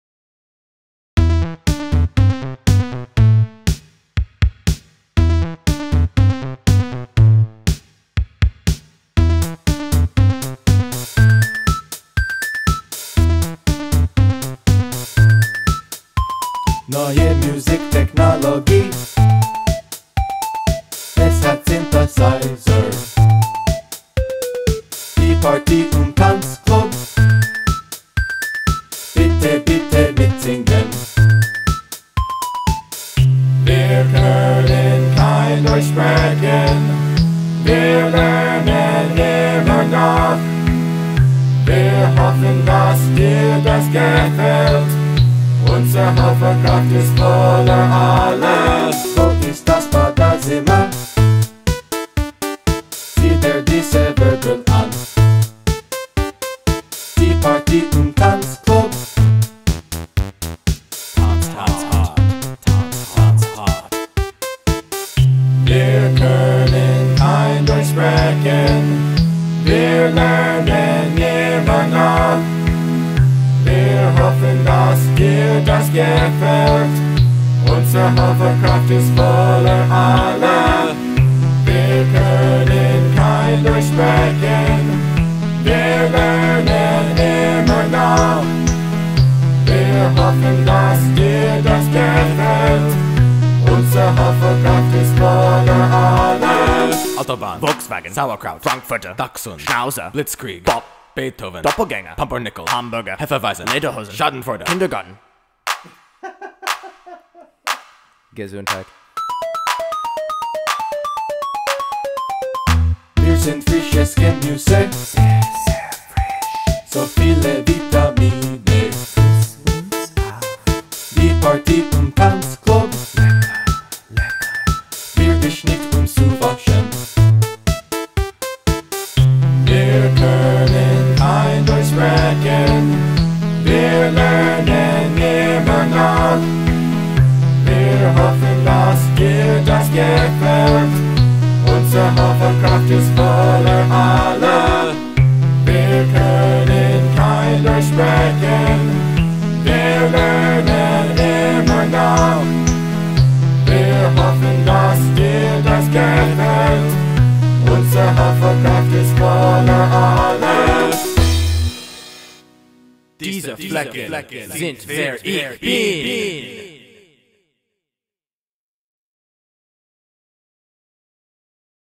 Die Party Im Tanzclub (Party in the Dance Club)
The story of a boy, Berlin, and some vegetables.